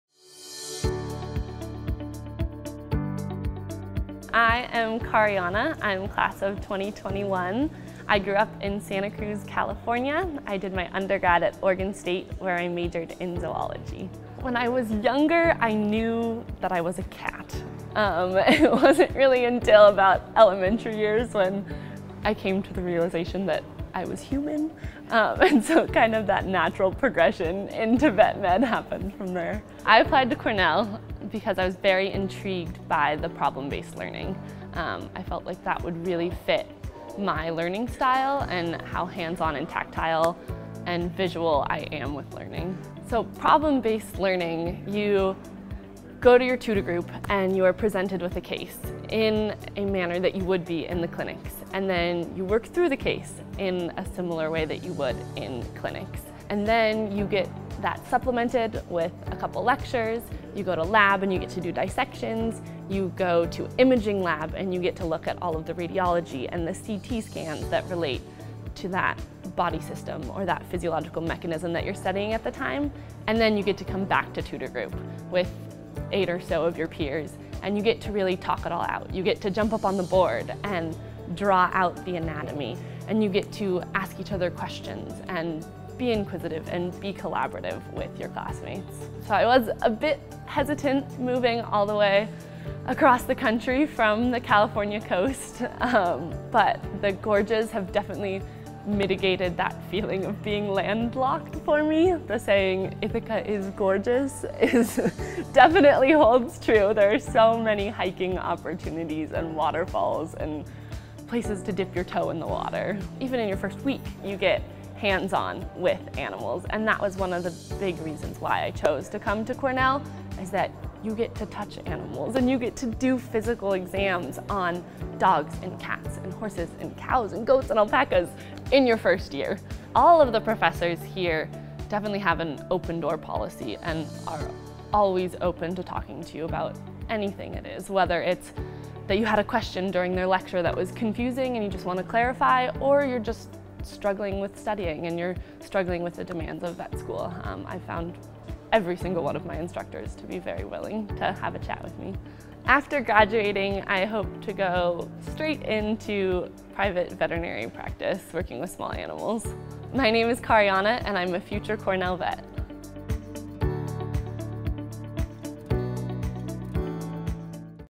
Hear from our students: Karianna Crowder, D.V.M. '21
Learn about life as a veterinary student at the Cornell University College of Veterinary Medicine from Karianna Crowder, D.V.M. '21.

Produced by White Knight Productions